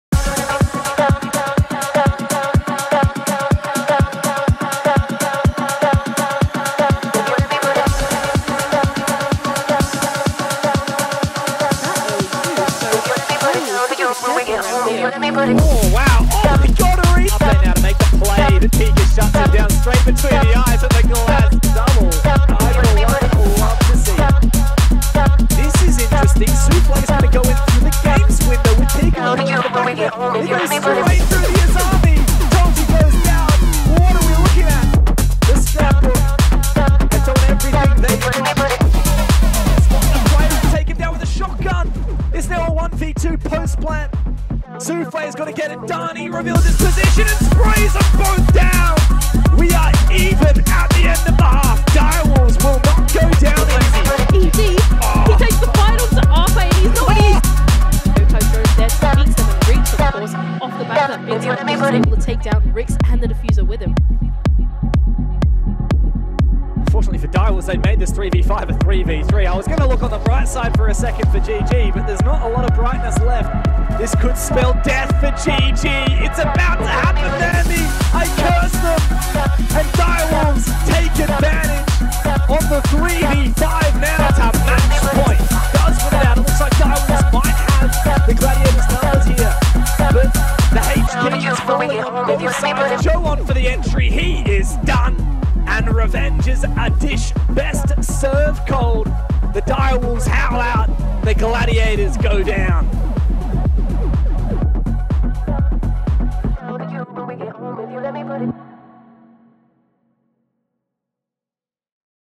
Wolfpack Run Wild! | APAC SOUTH STAGE 3 HLs vs Gaiman Gladiators
Start the week by catching up on our match with @GaiminGladiator 💪